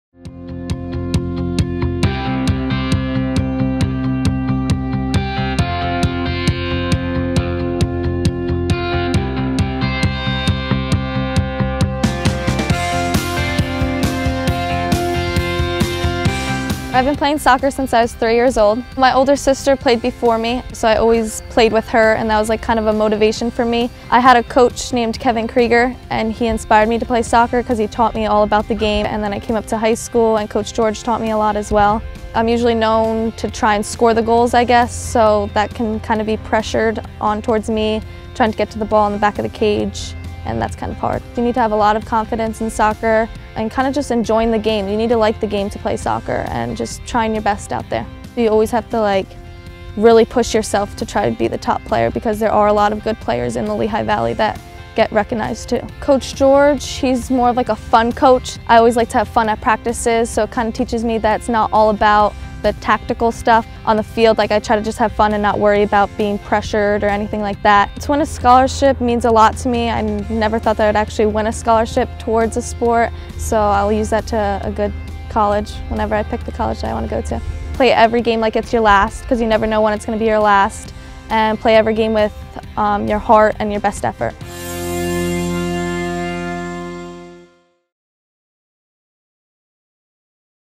Coordinated Health's Athlete of the Week - Kourtney Cunningham
Coordinated Health's Athlete of the Week 6 - Kourtney Cunningham - Whitehall High School Girls' Soccer - Attacking Midfielder